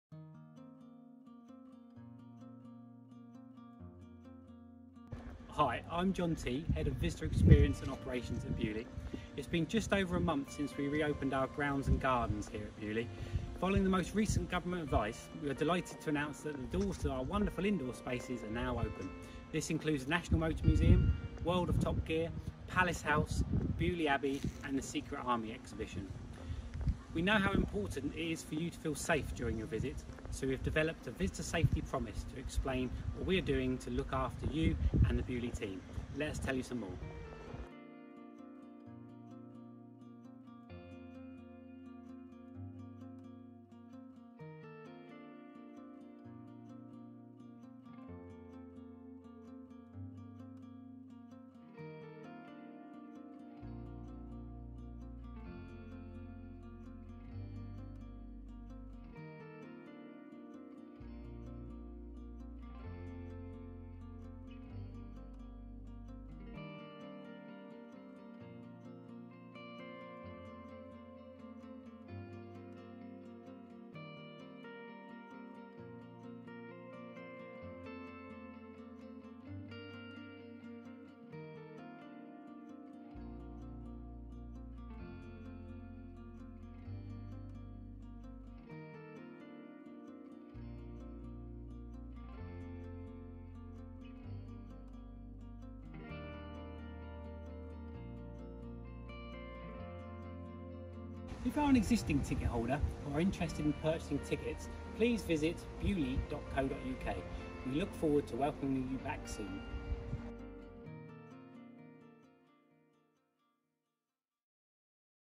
Beaulieu Visitor Safety Promise
We appreciate how important it is for you to feel safe during your visit to Beaulieu, so we have developed a Visitor Safety Promise to explain the steps we are taking to keep you safe during your day out at Beaulieu.

Beaulieu, Where marvellous days out are enjoy, together.